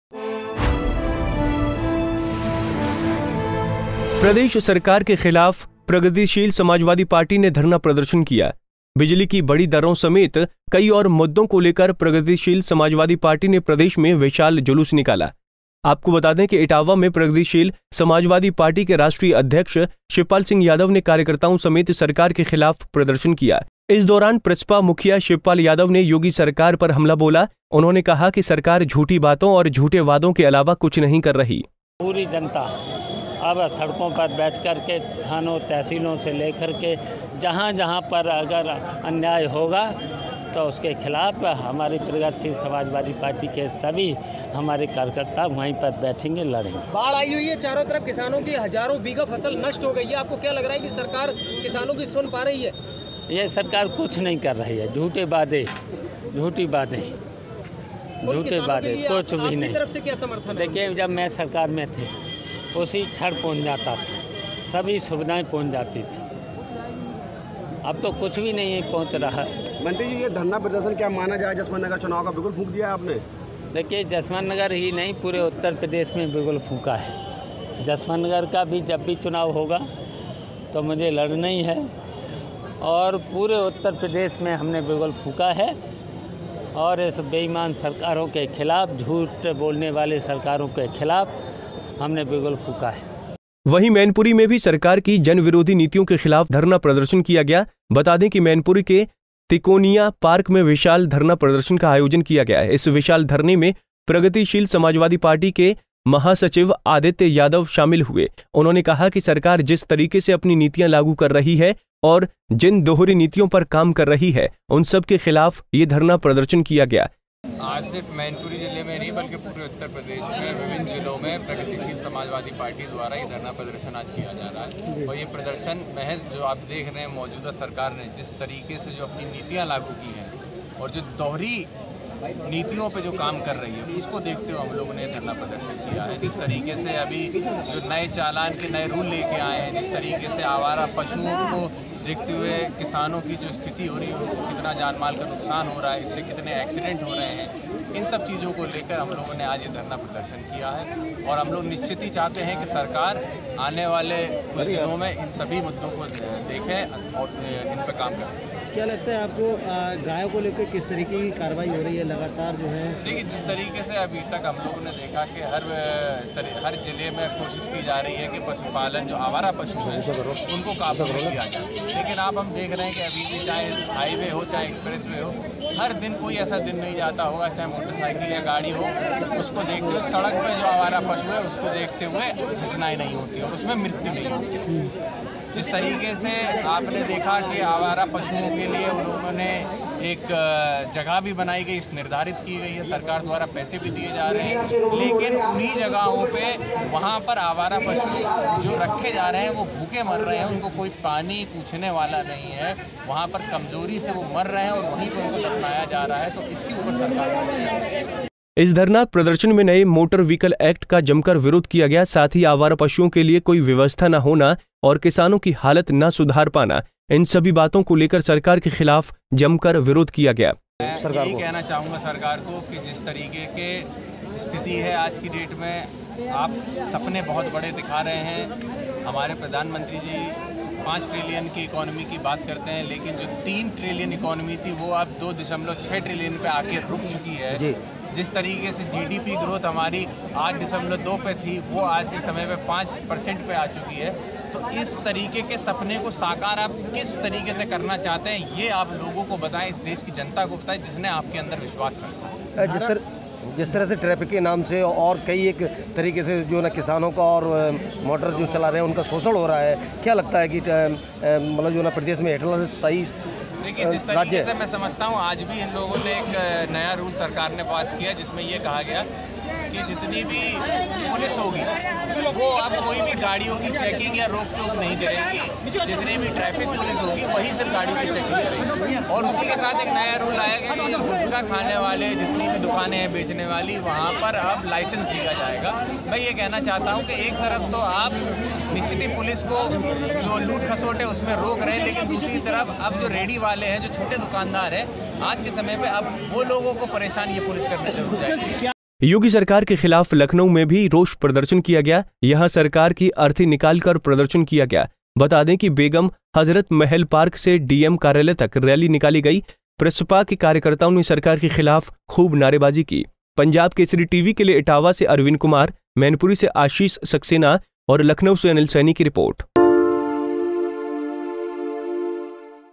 योगी सरकार के खिलाफ प्रगतिशील समाजवादी पार्टी का प्रदर्शन,धरने पर बैठे शिवपाल यादव
.बिजली की बढ़ी दरों समेत कई और मुद्दों को लेकर प्रगतिशील समाजवादी पार्टी ने प्रदेश में विशाल जुलूस निकाला. आपको बतादें कि इटावा में प्रगतिशील समाजवादी पार्टी के राष्ट्रीय अध्यक्ष शिवपाल सिंह यादव ने कार्यकर्ताओं समेत सरकार के खिलाफ प्रदर्शन किया.To Subscribe on Youtube